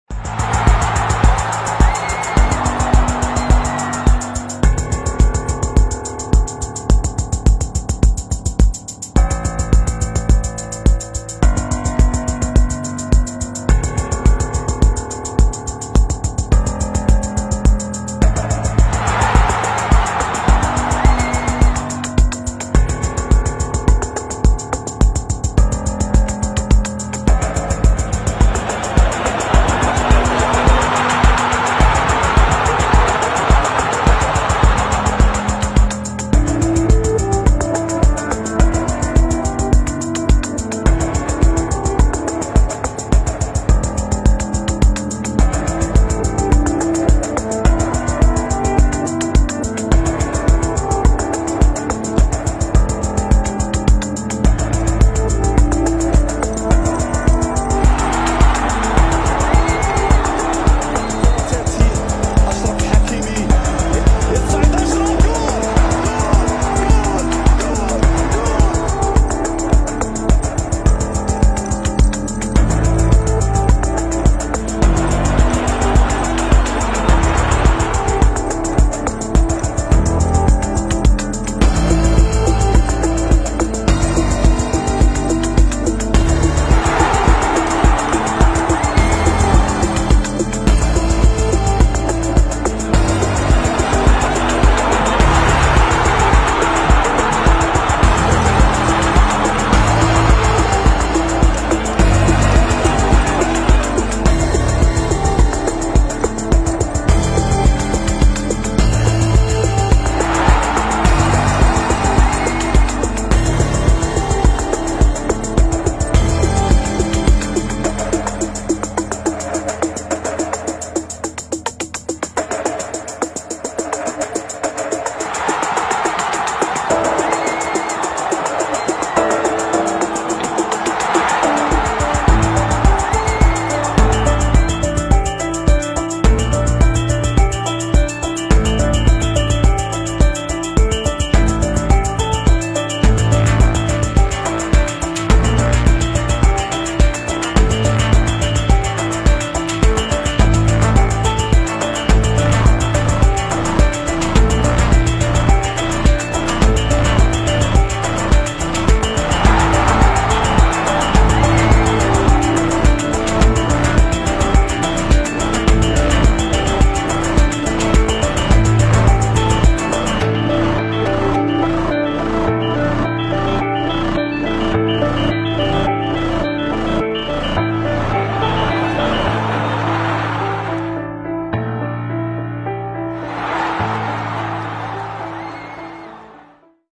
Achraf Hakimi e sua polêmica comemoração contra Luis Enrique
“Luis Enrique cometeu um erro grave”. O Marrocos foi a grande surpresa da Copa do Mundo ao derrotar a Espanha nos pênaltis. O herói do dia foi o marroquino Achraf Hakimi, autor de um golaço na vitória épica dos africanos. Mas o zagueiro do PSG fez uma comemoração polêmica e mandou um recado forte ao técnico espanhol, Luis Enrique. O “espanhol” Hakimi vingou um amigo seu e também destruiu os sonhos de seu país de nascimento.